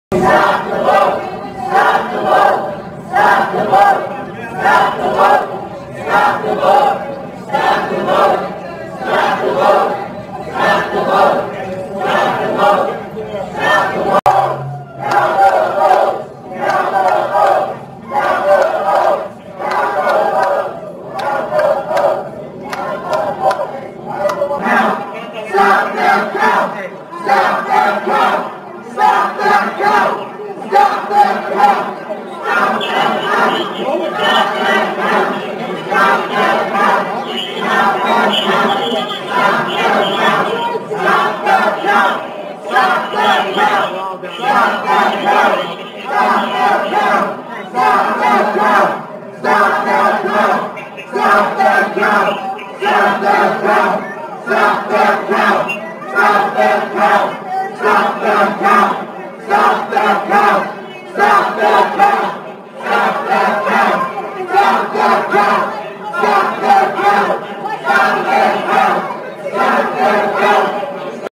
Stop the count! Vs. Count those Votes! [Contradiction 101]
Trump supporters yelling stop the count! Vs the ones who want the votes to be counted.
You gotta love 'em.